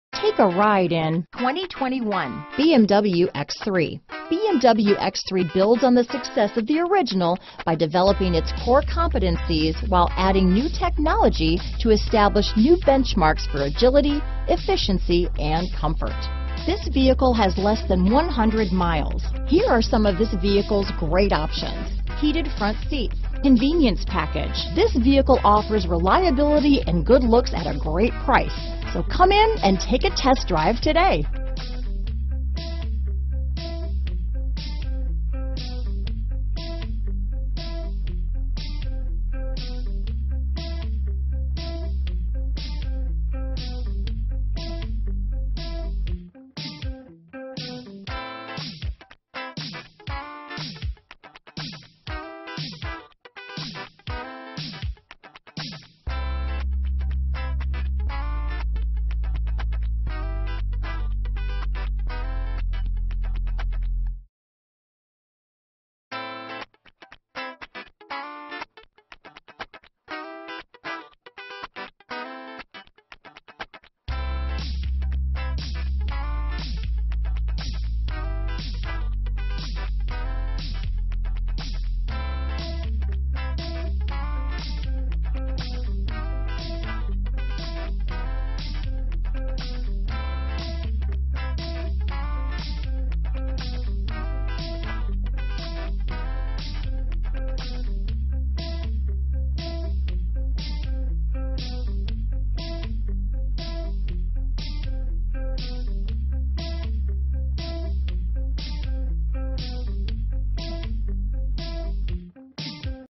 2021 BMW X3 Tuscaloosa, Birmingham, Greensboro, Bessemer, AL, Columbus, MS 9D87360
Glacier Silver Metallic New 2021 BMW X3 available in Tuscaloosa, Alabama at BMW of Tuscaloosa. Servicing the Birmingham, Greensboro, Bessemer, Columbus, MS area.

BMW of Tuscaloosa presents this 2021 BMW X3 sDrive30i Sports Activity Vehicle in Glacier Silver Metallic with Canberra Beige interior.brNicely equipped with Convenience Package (Comfort Access Keyless Entry, Panoramic Moonroof, Lumbar Support), Heated Front Seats,and much more!brbrCall Today to Schedule your VIP Appointment: .
Convenience Package,Heated Front Seats